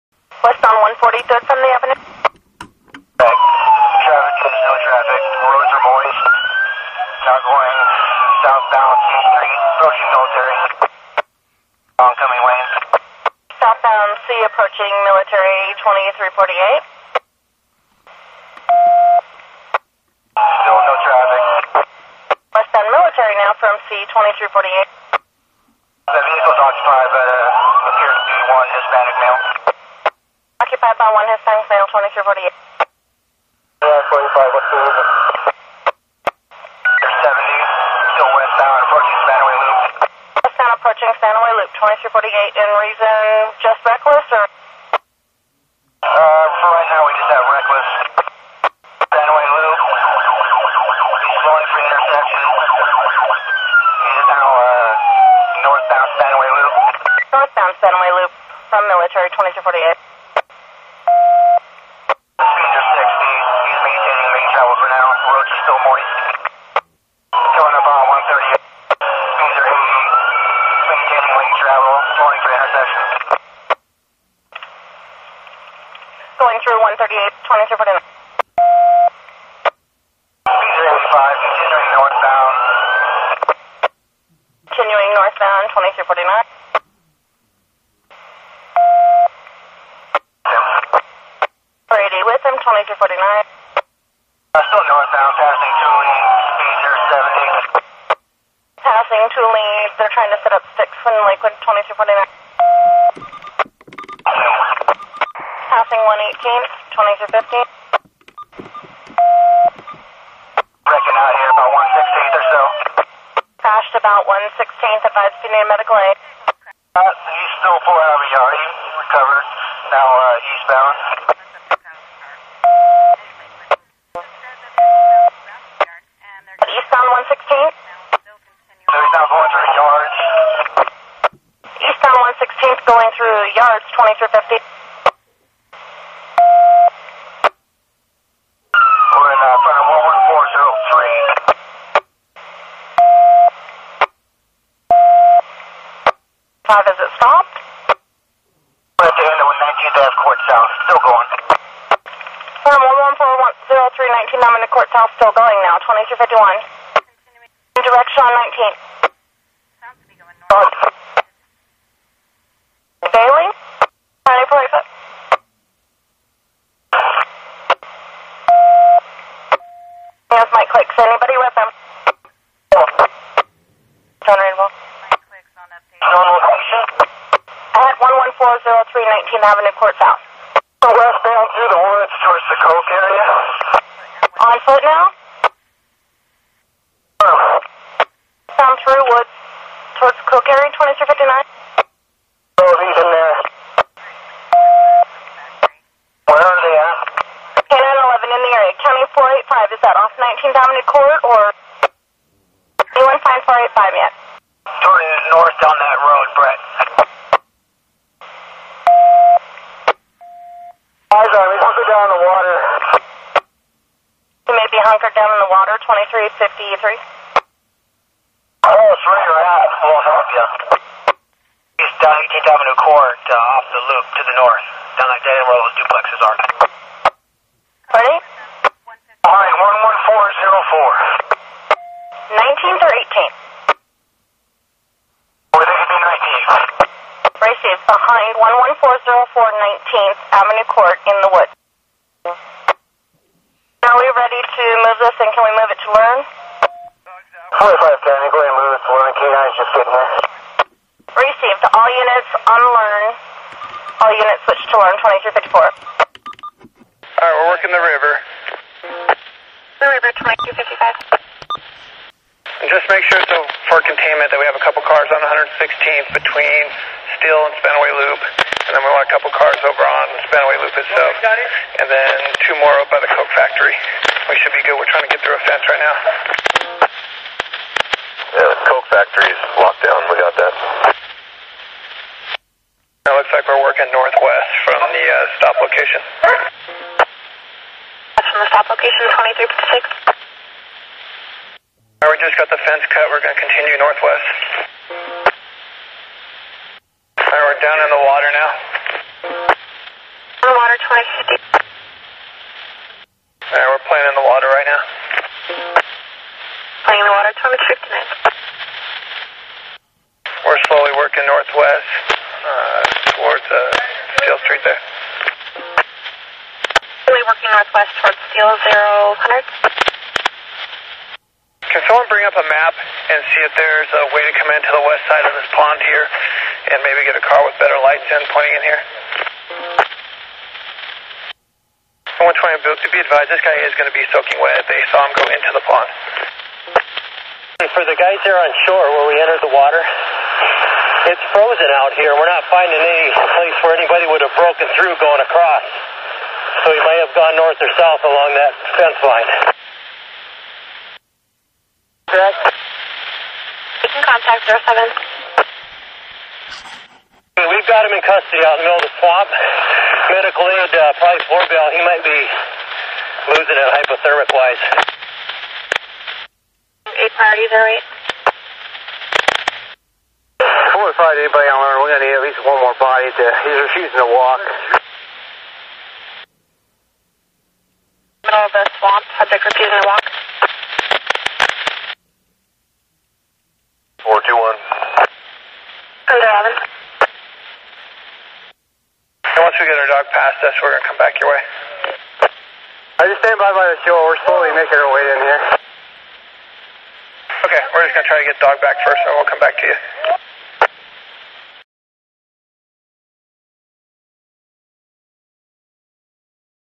Pierce County Sheriffs Department vehicle pursuit and foot chase into pond scanner recording
(1-23-12 at 11:48pm) The Pierce County Sheriffs Department in pursuit of a vehicle that fails to yield and the suspect ends up bailing and runs into a pond, only to be caught by Lakewood PD K9.

The Vehicle stopped in the 1900 Block of Spanaway Loop Rd.
(From Pierce County, Washington)